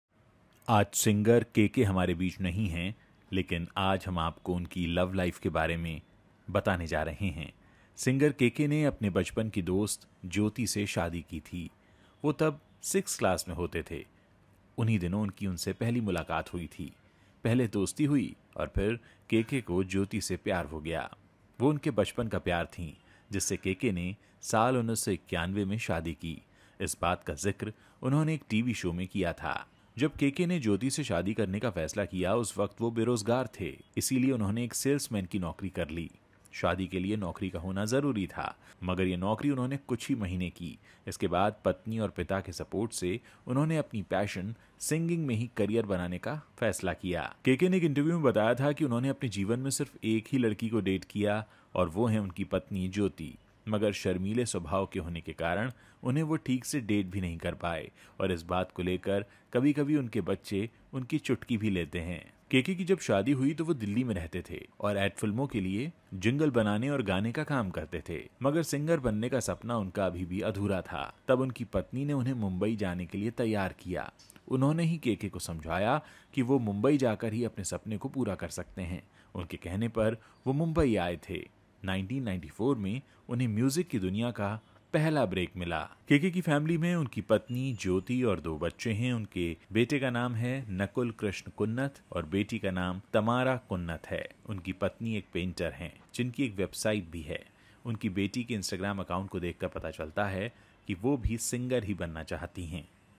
Singer K K की Love Story | जानिये क्या ख़ास था उनकी प्रेम कहानी में
Google Play Store Link for my App

Host :- Ujjawal Trivedi
Copyrights & Label :- Ujjawal Trivedi